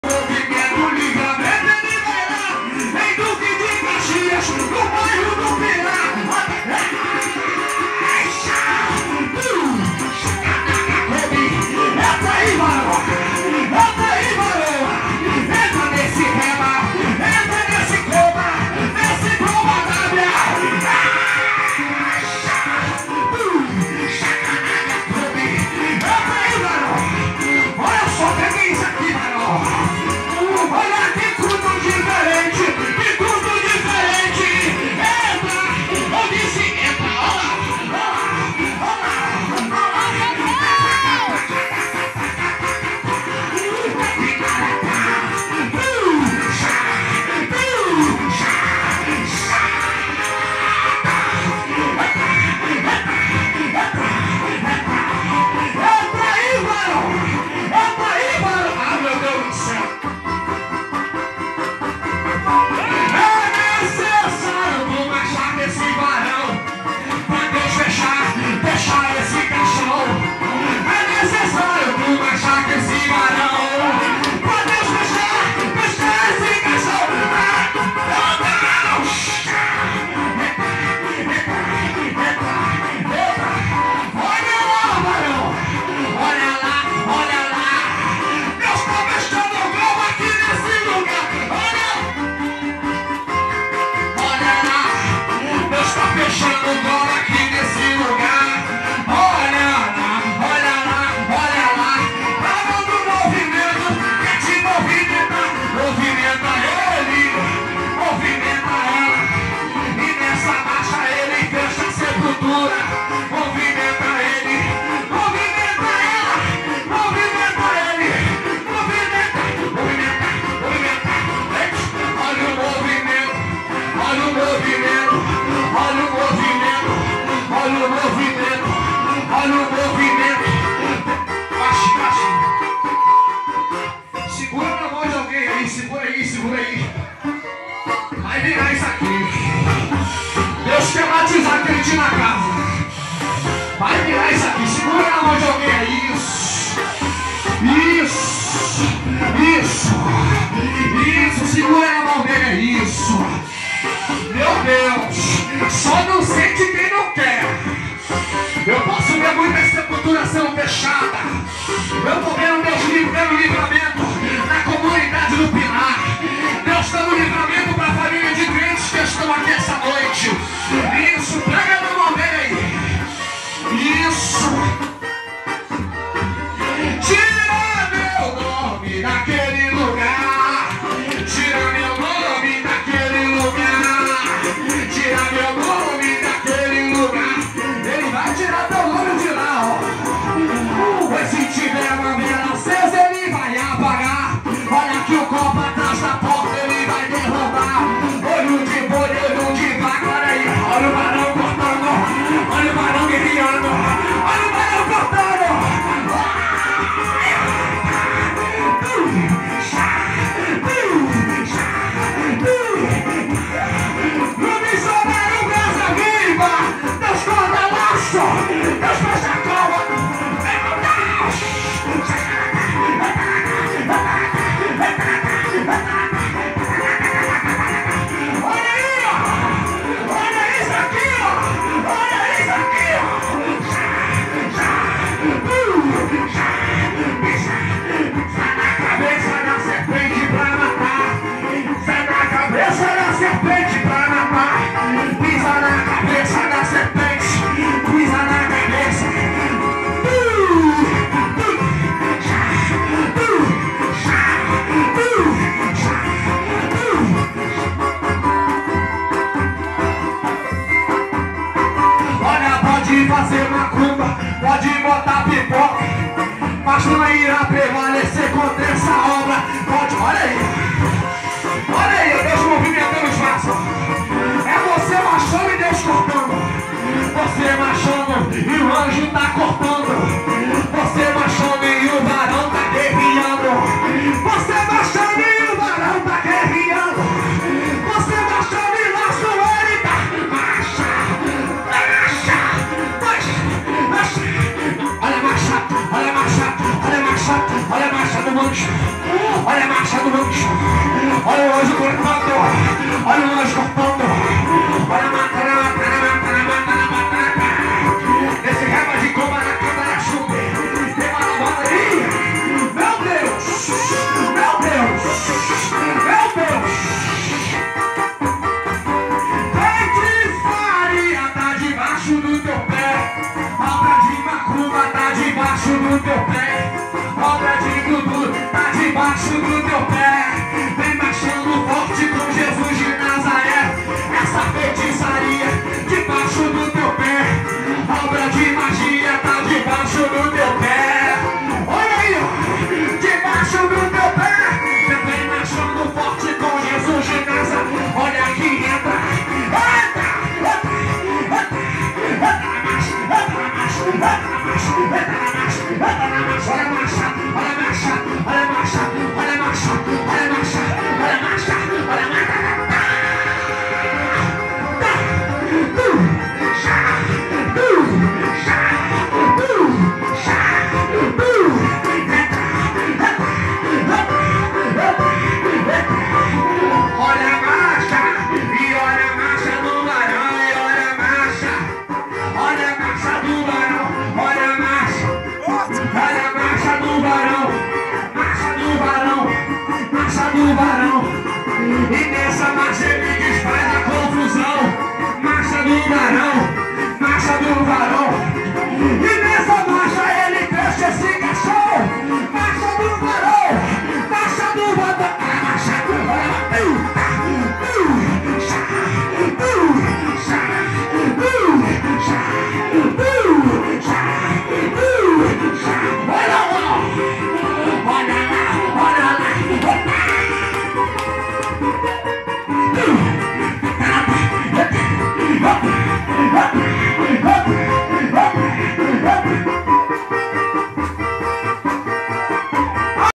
CANTOR FELIPE OLIVEIRA DEU LUGAR NO MINISTÉRIO BRASA VIVA
O levita felipe Oliveira deu lugar na nossa sede AD MINISTÉRIO MISSIONÁRIO BRASA VIVA
Corinhos de fogo